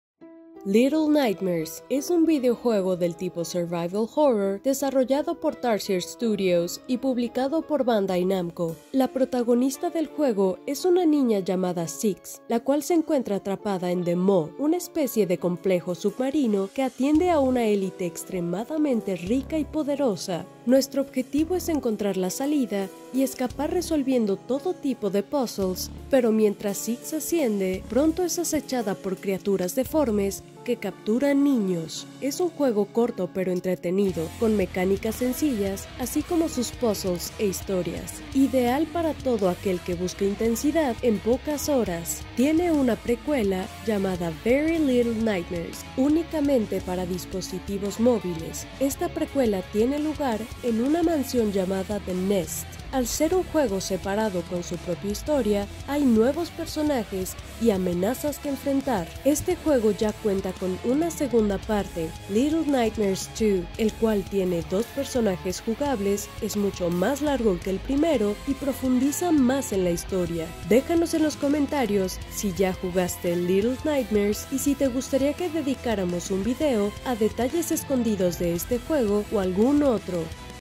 Curiosidades sobre el videojuego Little Nightmares
Little Nightmares es un videojuego del tipo survival horror desarrollado por Tarsier Studios y publicado por Bandai Namco, te invitamos a conocer más sobre este juego y activa la campanita para que te enteres cuando publiquemos nuevos vídeos.